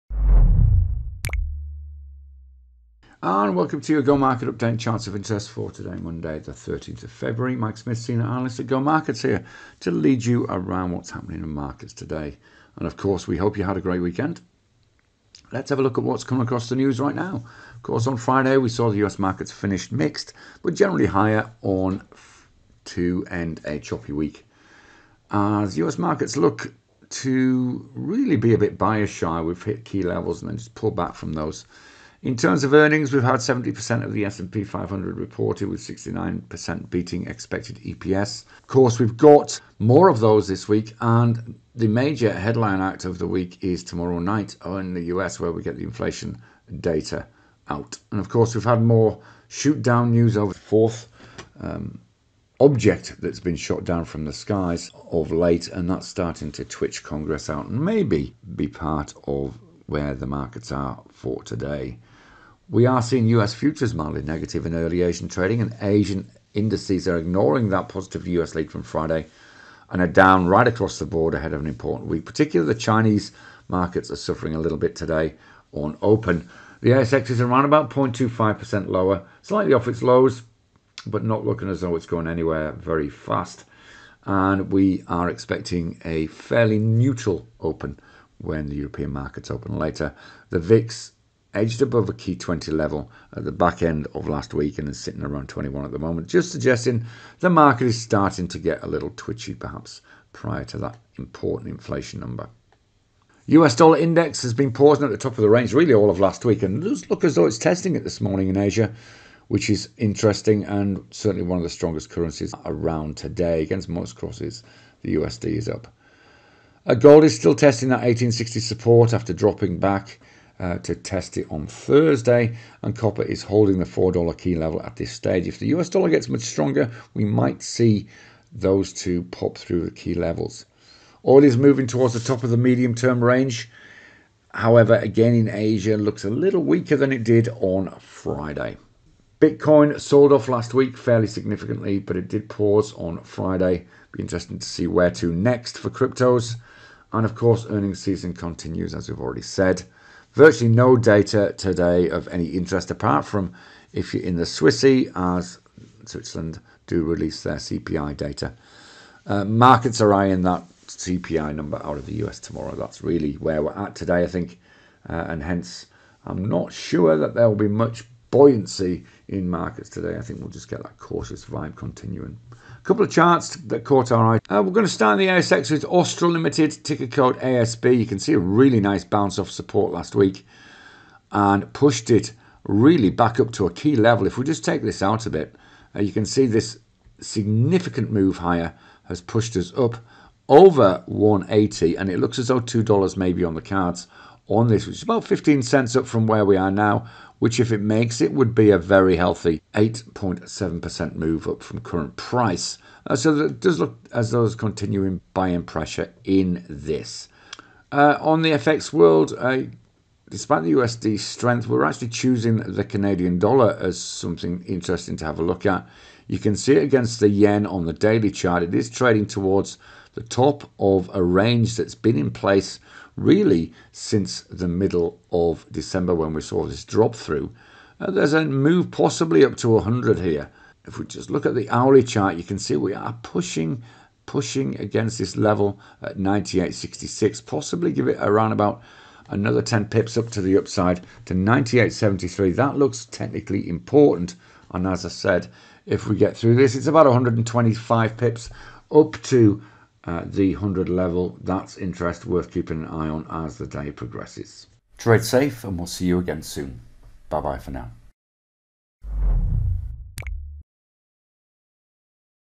GO Market Update and Charts of the day CADJPY and ASB
Markets bracing for more earnings and a BIG US inflation number out tomorrow. Find out more.

 人工客服小助手微信：gomarketssyd 或 gomarketsau5
墨尔本电话（中文服务）：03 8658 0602
悉尼电话（中文服务）：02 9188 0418